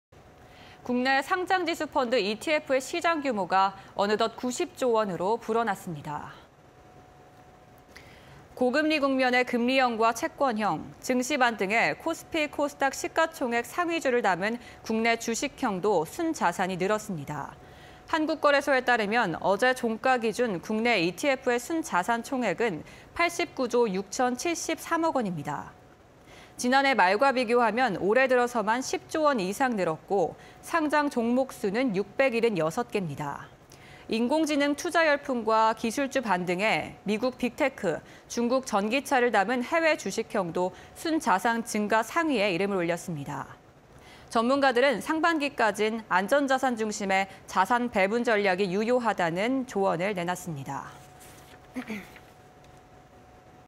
ETF 90조 시대... 올해 금리형·채권형 순자산 '쑥' (20230215)
국내 상장지수펀드(ETF) 시장 규모가 어느덧 90조원으로 불어났습니다.

고금리 국면 금리형과 채권형, 증시 반등에 코스피·코스닥 시가총액 상위주를 담은 국내 주식형도 순자산이 늘었습니다.

한국거래소에 따르면 어제(14일) 종가 기준 국내 ETF 순자산총액은 89조6073억원입니다. 지난해 말(78조5116억원)과 비교하면 올해 들어서만 10조원 이상 늘었고, 상장종목수는 676개입니다.

인공지능(AI) 투자 열풍과 기술주 반등에 미국 빅테크, 중국 전기차를 담은 해외 주식형도 순자산 증가 상위에 이름을 올렸습니다.

전문가들은 상반기까진 안전자산 중심의 자산배분 전략이 유효하다는 조언이 내놨습니다.